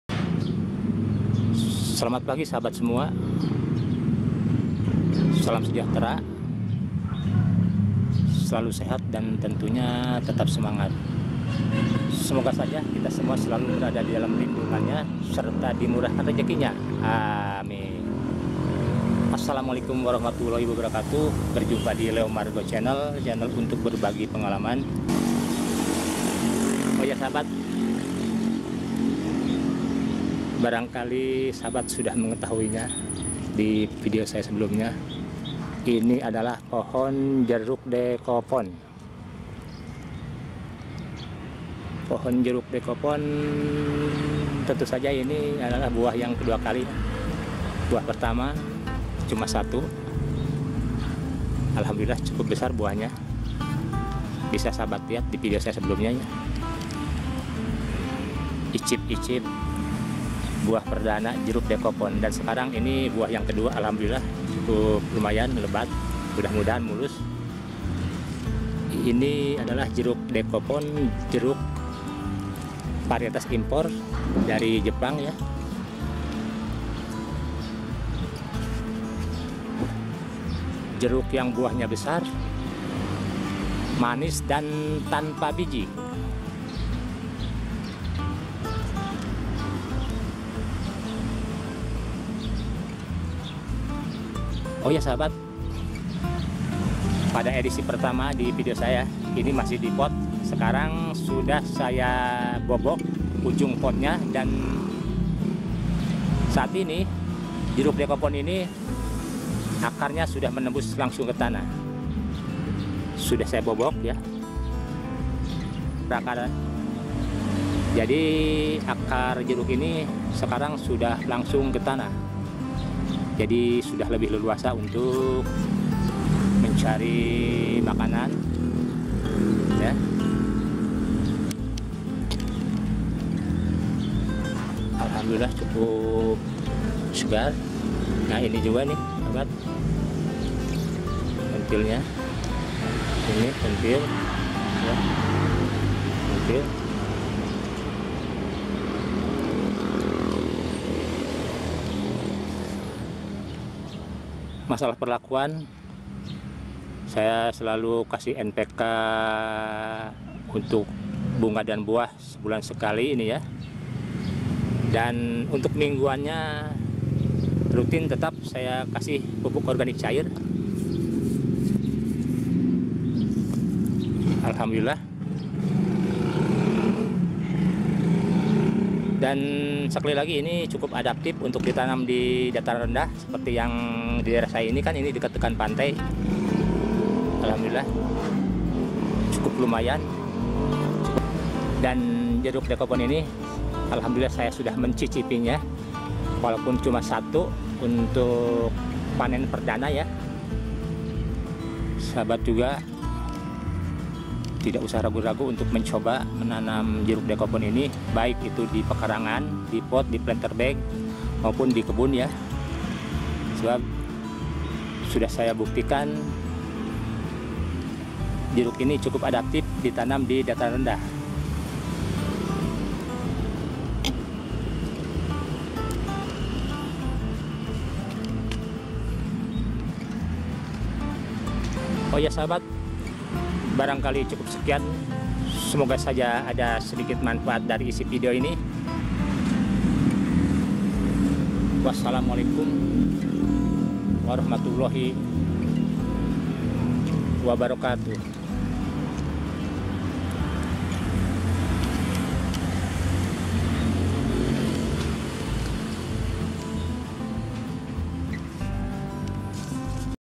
JERUK DEKOPON TERBUKTI ADAPTIF DI DATARAN RENDAH...
Meskipun jeruk Dekopon merupakan jeruk varietas import asal Jepang sebagai tanaman sub tropis - yang buahnya cukup besar, manis dan tanpa biji. Terbukti Adaptif  untuk ditanam di dataran rendah dan berhawa panas. Bisa sahabat tanam di pekarangan baik di pot, di planterbag, langsung di tanah maupun dikebunkan. Asal perawatan dan pemupukannya. Selamat mencoba bagi yang belum...